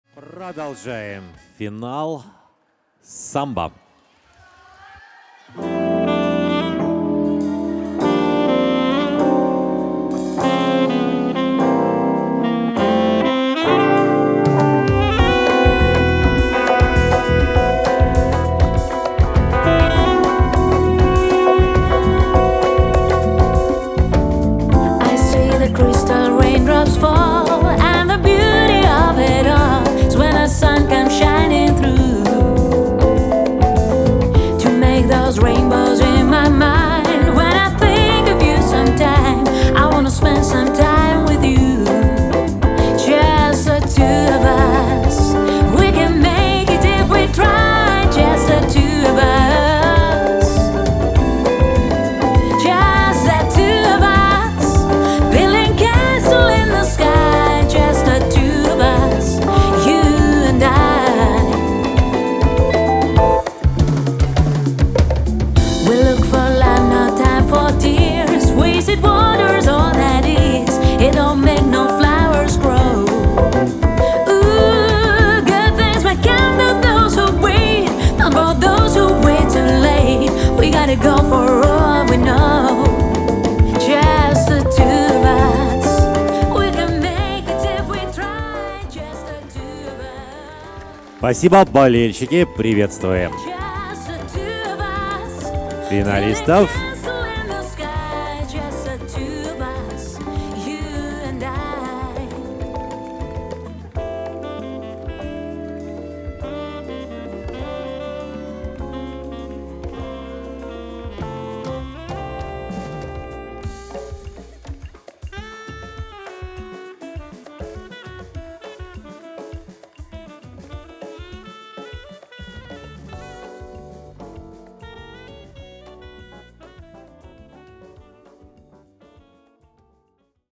☂Молодежь 2(до 21 лет)(Open)Samba(S)ФиналLA(5)ROYAL BALL–2021 Cоревнования по спортивн бальн танцам
ROYAL BALL – 2021 ОТКРЫТЫЕ РЕСПУБЛИКАНСКИЕ СОРЕВНОВАНИЯ ПО СПОРТИВНЫМ БАЛЬНЫМ ТАНЦАМ
дата проведения: 30-31 января 2021 года
Организаторы:
Место проведения: ЧИЖОВКА-АРЕНА, г. Минск, ул Ташкентская 19
3 отделение 19:00 31 января

Финал

пара №443 Poleshchikov Vladislav & Yakovleva Veronika - 1 место
(Club: USSR, г. Москва, Russia)
Тренер(ы): Syzrantcev Dmitriy & Syzrantceva Alen

пара №433 Kanaluk Nichita & Yurova Alina - 2 место
(Club: Royal Dance Team, Odessa Ukraine)
Тренер(ы): Gotsulyak Oleksii

пара №444 Герасименок Кирилл и Давидович Александра - 3 место

пара №446 Михальченко Никита и Духновская Ульяна - 4 место

пара №479 Чижик Святослав и Лепко Вероника - 5 место

пара №380 Майданюк Михаил и Снегирь Анна - 6 место

пара №509 Челешкин Алексей и Шевченко Злата - 7 место